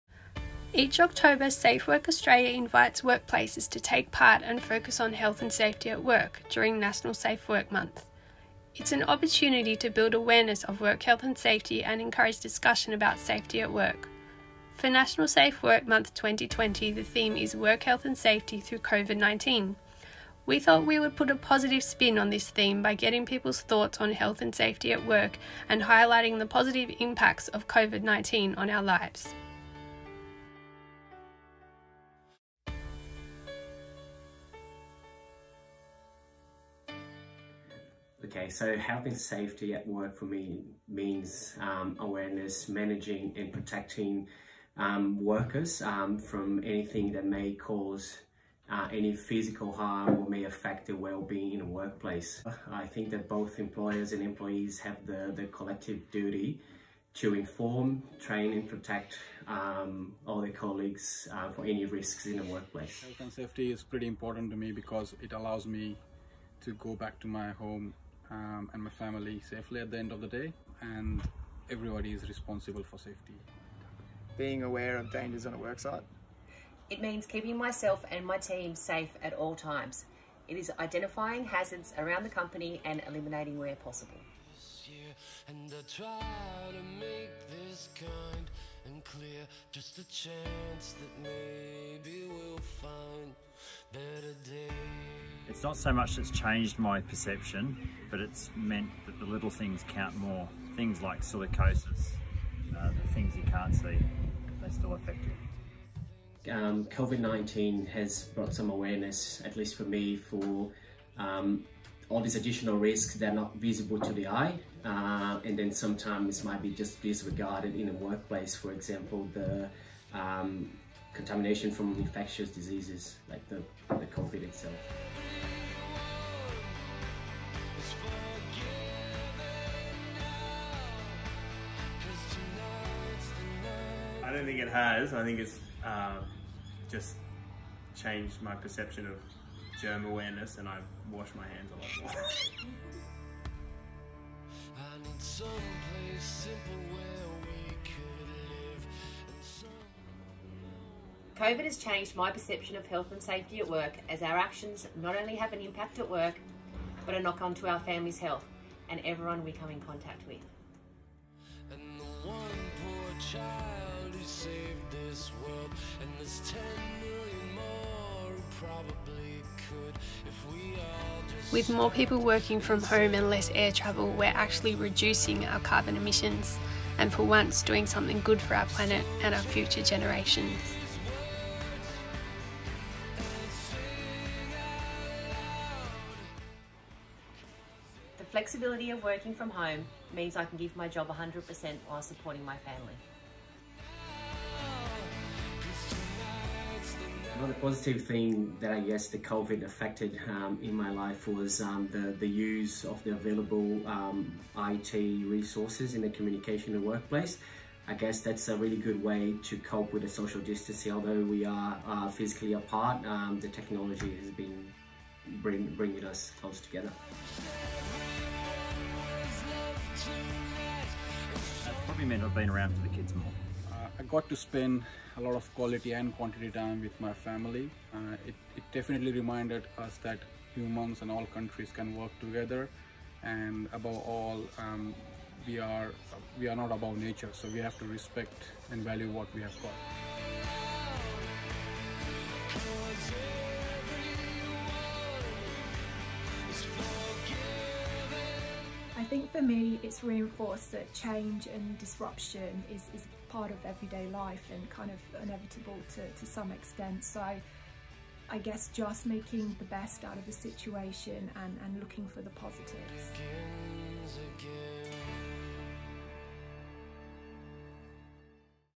National Safe Work Month 2020
Each October, Safe Work Australia invites workplaces to take part and focus on Health and Safety at work during National Safe Work Month. It's an opportunity to build awareness of Work Health and Safety and encourage discussion about safety at work.
For National Safe Work Month 2020 the theme is WHS through COVID-19.
We thought we would put a positive spin on this theme by getting people's thoughts on Health and Safety at work and highlighting the positive impacts of COVID-19 on our lives.